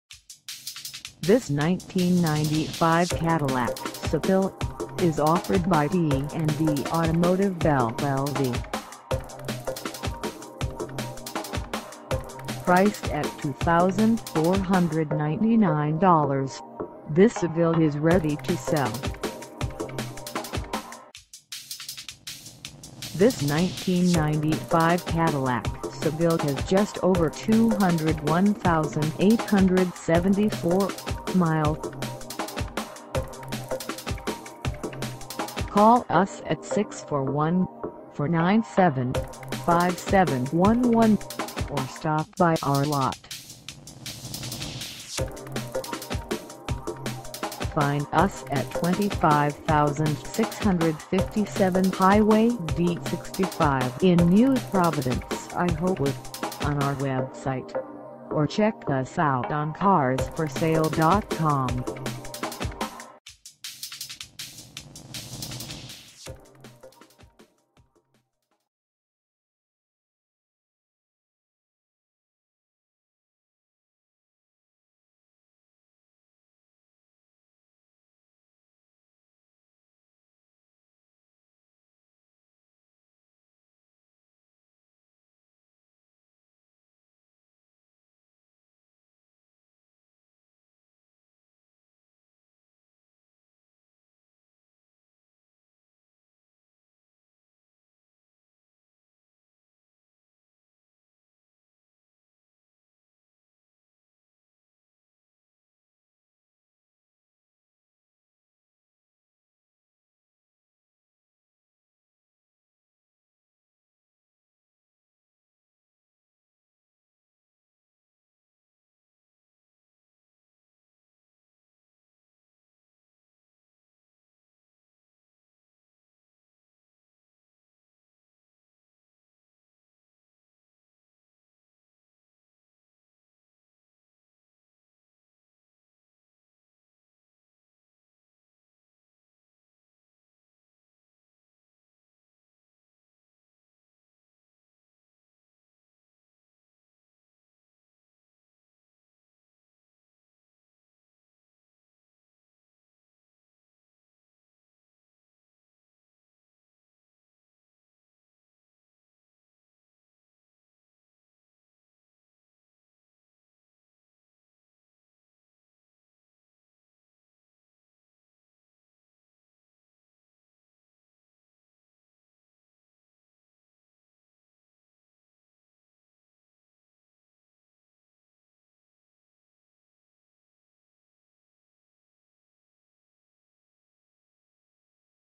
1995 Cadillac Seville SLS - for sale in NEW PROVIDENCE, IA 50206
D & D AUTOMOTIVE LLC
25657 HIGHWAY D65

Come test dirve this 1995 Cadillac Seville SLS for sale in NEW PROVIDENCE, IA.